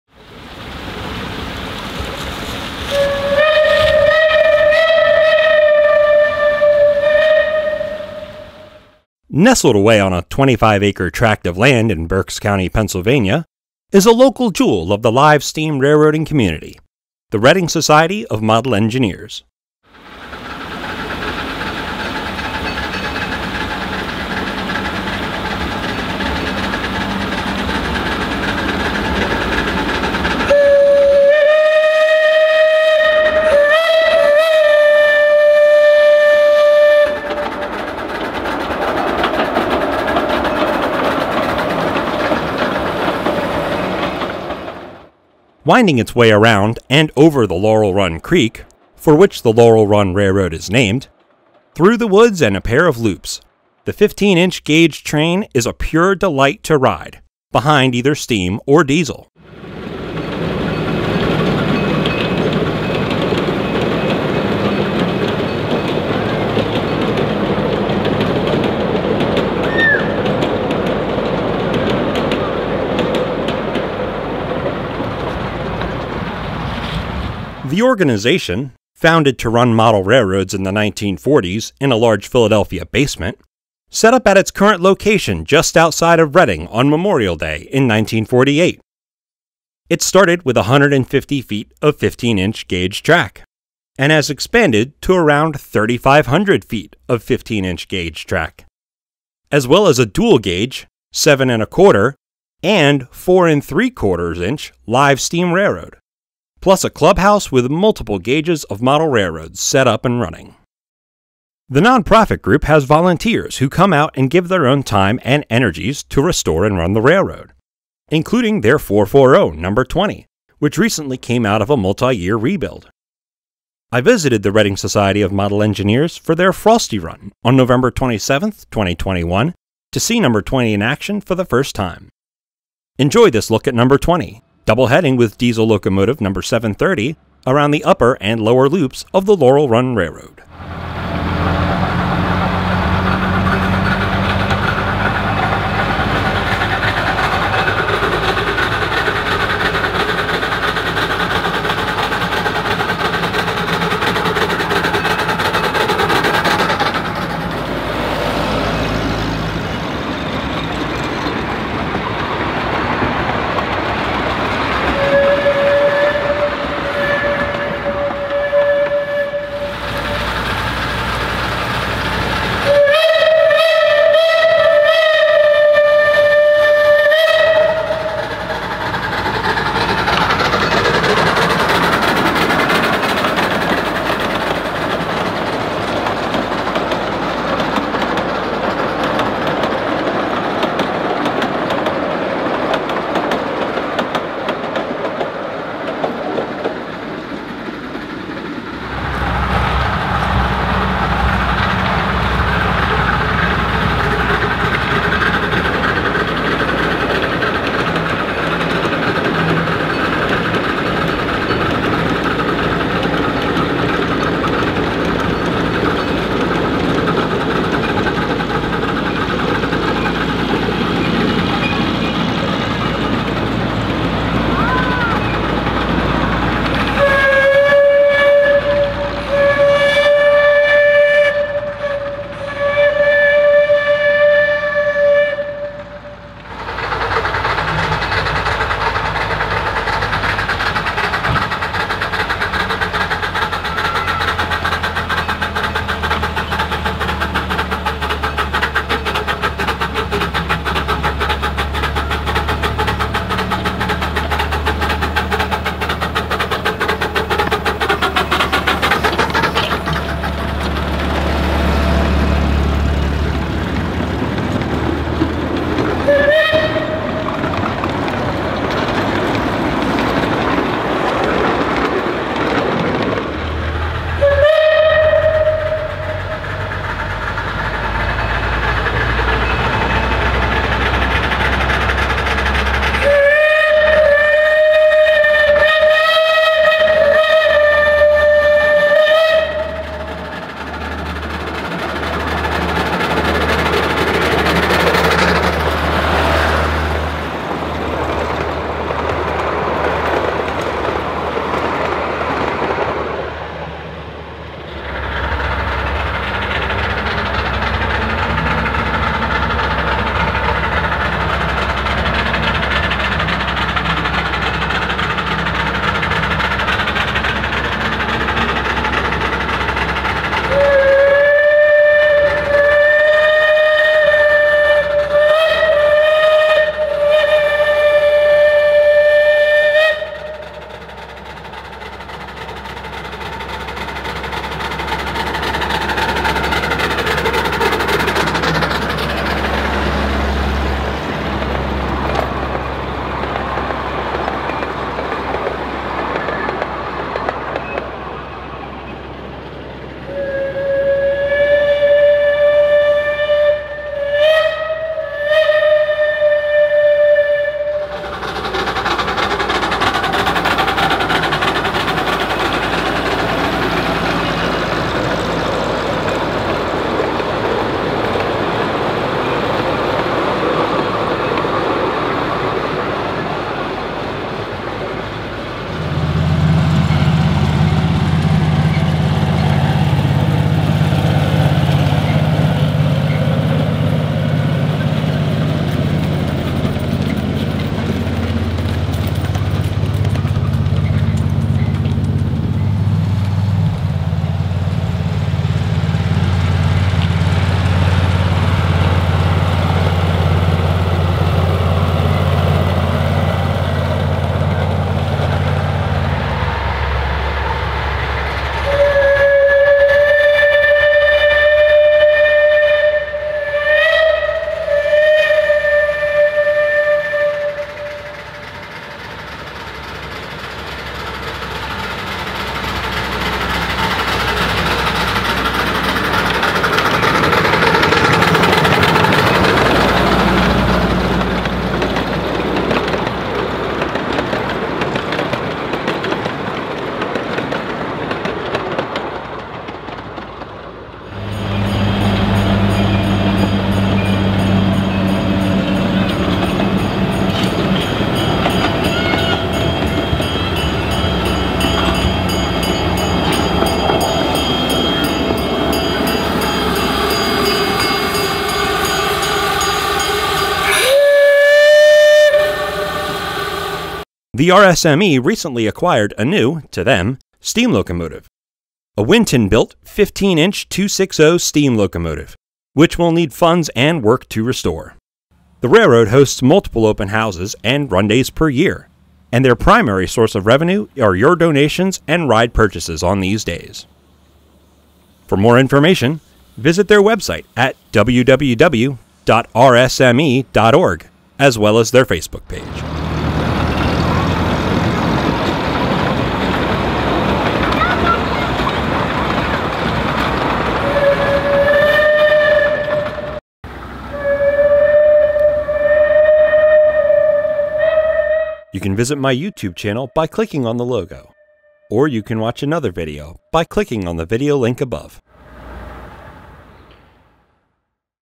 Reading Society of Model Engineers: Frosty Run 2021
On a 25-acre tract of land, outside of Reading, PA, you'll find an amazing group of people who make up the Reading Society of Model Engineers (RSME). The RSME operates, among other things, the Laurel Run Railroad - a 15" gauge live steam railroad that loops and turns through the hills of Berks County.  I made a visit to the RSME for their Frosty Run event, on November 27, 2021, to film and ride behind their recently-repaired 4-4-0 #20 steam locomotive.  But that's not all the RSME has to offer!!  In this video, we'll take a quick look at the RSME as a whole - and hopefully you'll come out to visit during one of their open houses or run days in the near future!  So enjoy this look at the Reading Society of Model Engineers:  Frosty Run 2021!!

(All photos either provided by RSME's Facebook page, or are my own photos.)